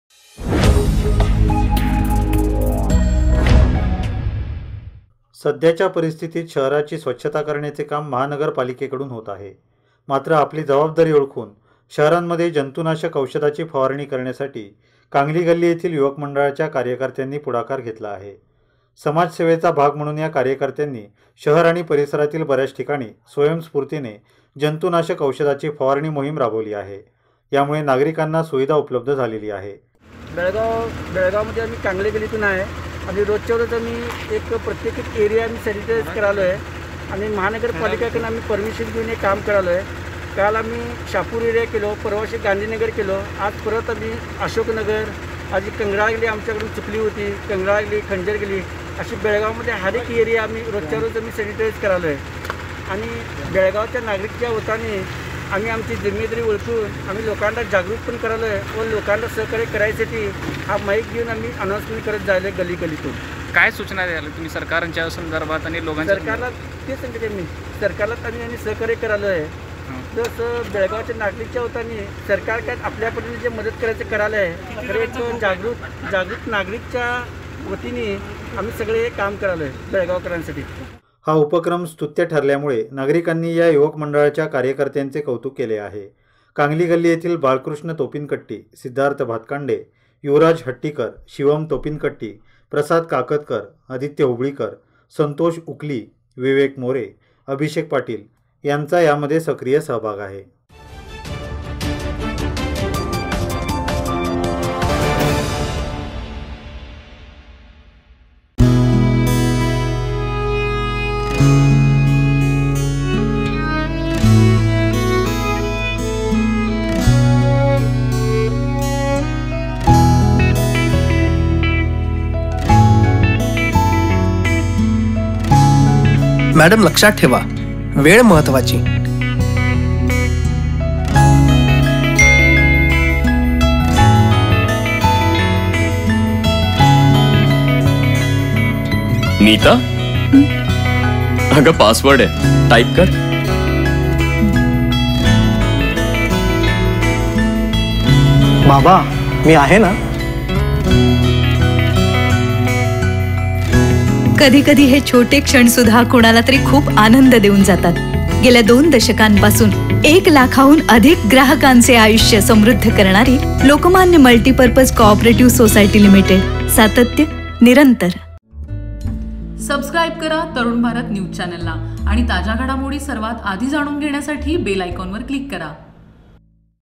कांगली गल्ली मंडळाचा स्तुत्य उपक्रम | Belgaum News | 28-03-2020
जंतूनाशक औषध फवारणी करण्यासाठी कांगली गल्लीतील युवकांचा पुढाकार
 शहर आणि परिसरातील बऱयाच ठिकाणी केली औषध फवारणी

Ads :lokmanya society